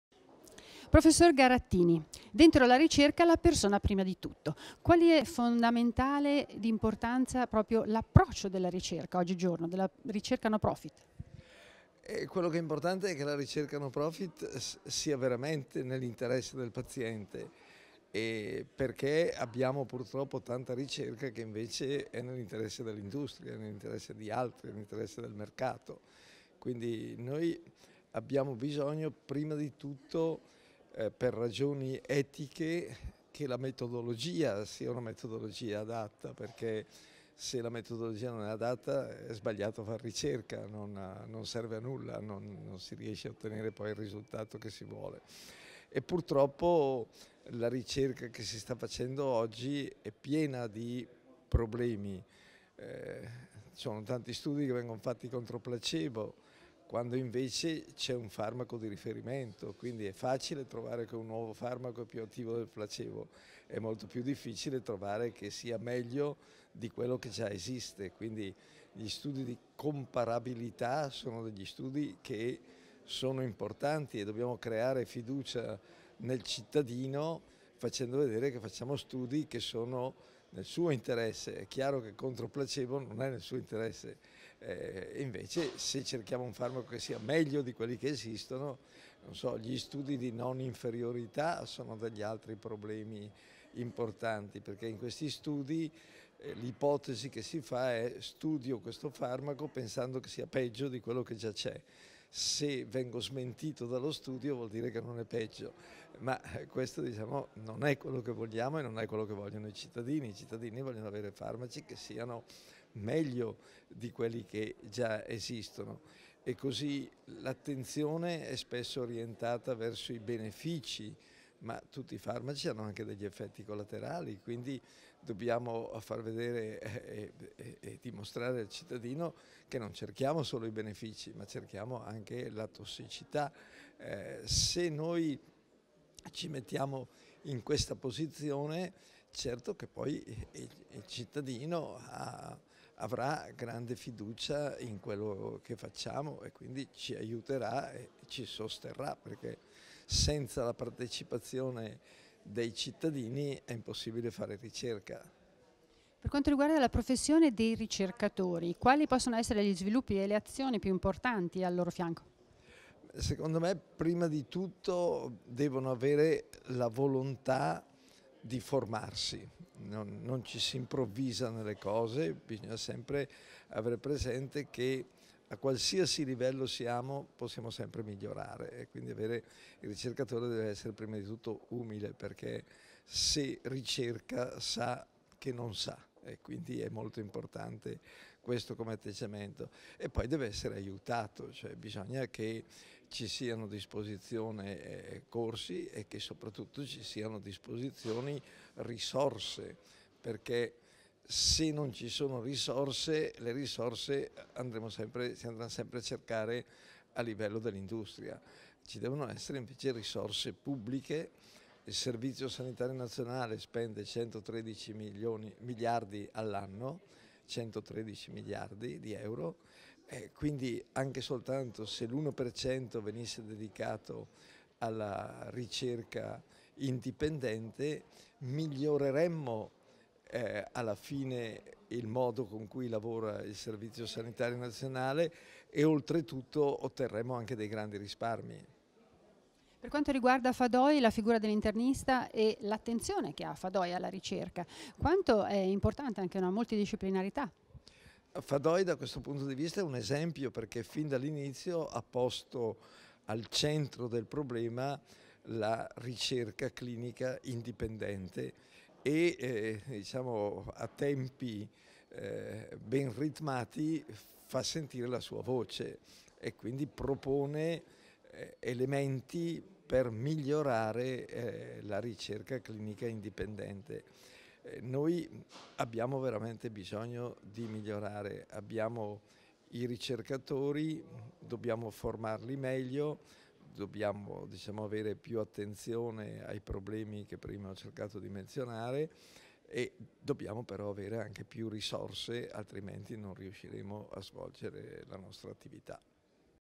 Intervista al prof. Garattini, dal 5° Convegno Nazionale Ricerca FADOI
La persona prima di tutto, e non nell'interesse dell'industria